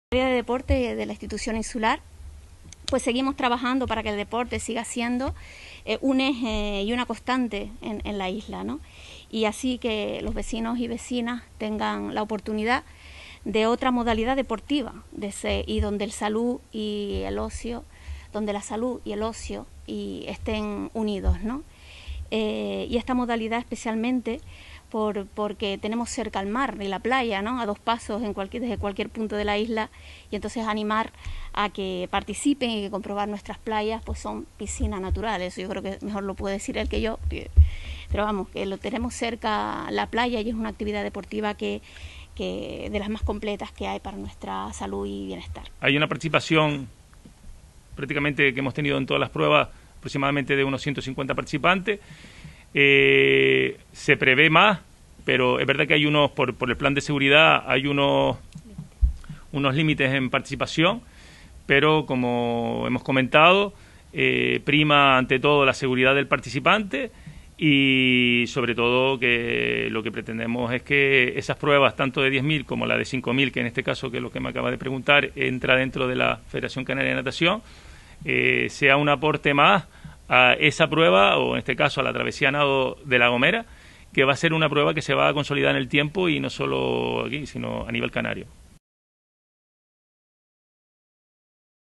La Gomera se prepara para vivir su segunda Travesía a Nado
Video de Oliver